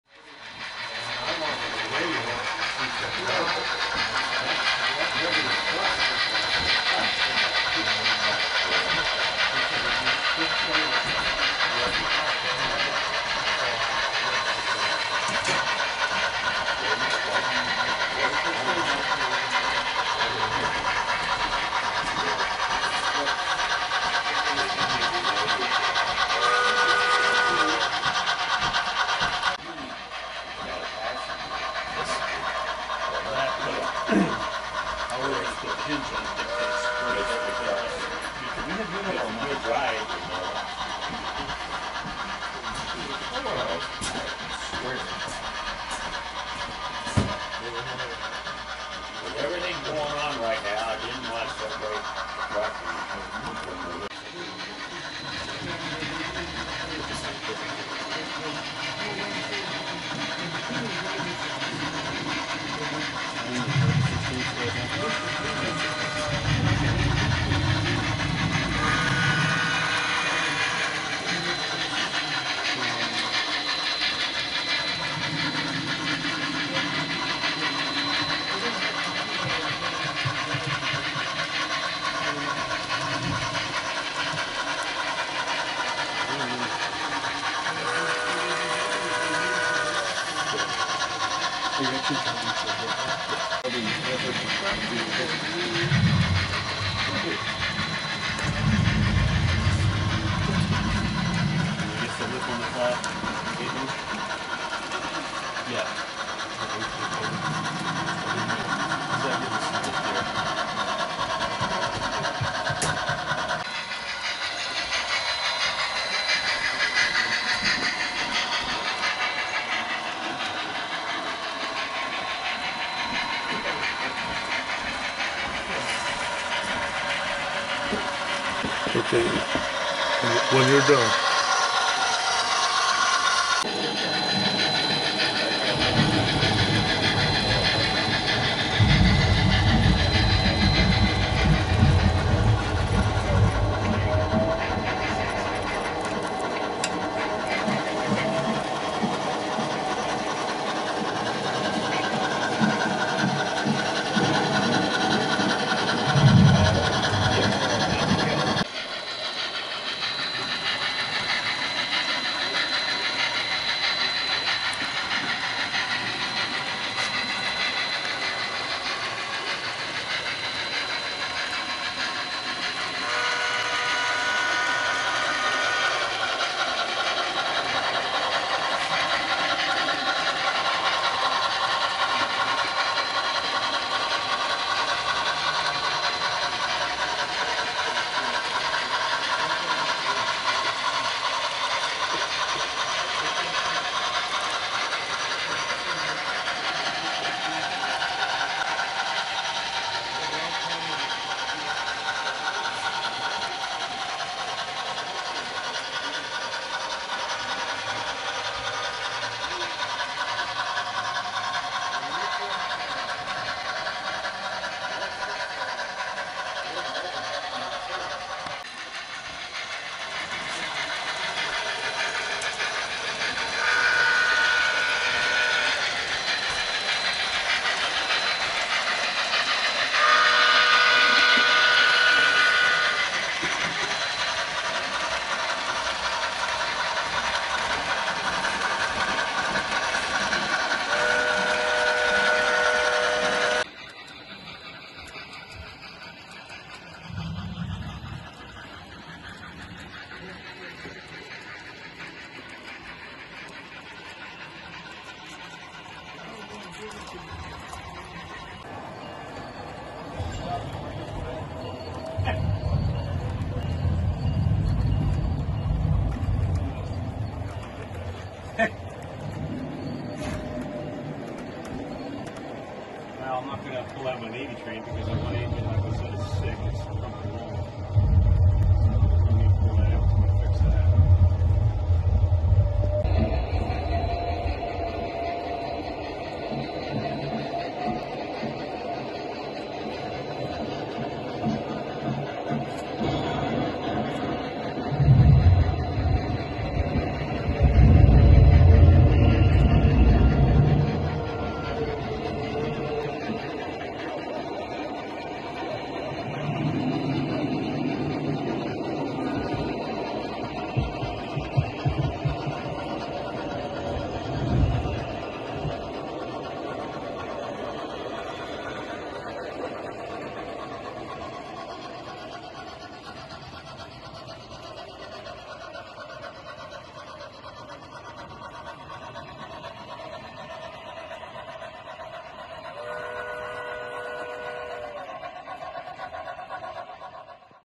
Sunday run on the ESRS
Sunday, Nov. 3rd. 2024
Having fun running trains on the ESRS layout.
Yes, well aware of the blooper. Turnout was thrown, casing derailment.
 It happens. ;)